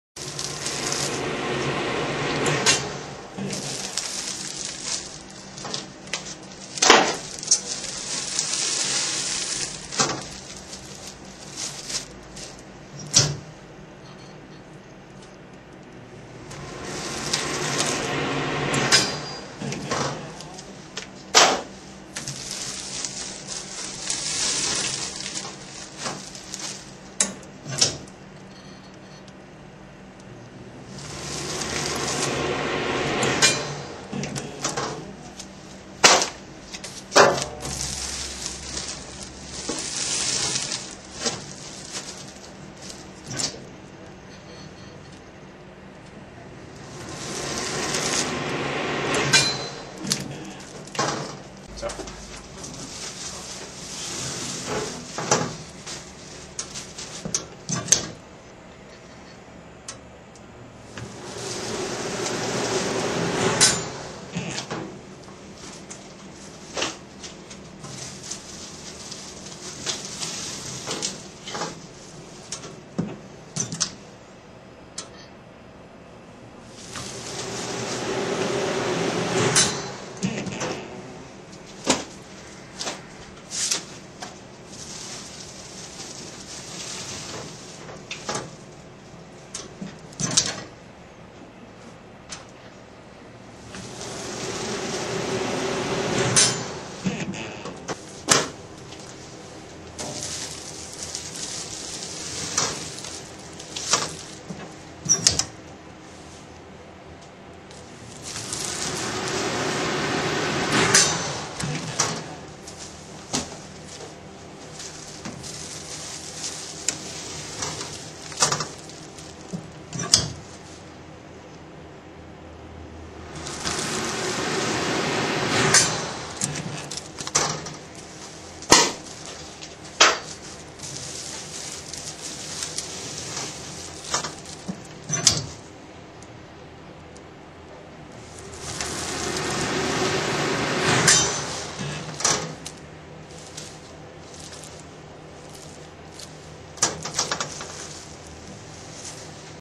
Entry Level Shrink Wrap Machine
The S3 Chamber Wrapper is a single step system for shrink wrapping products.

**Update:  We no longer carry this specific machine.  Please see our new line of Shrink Wrapping products on our website**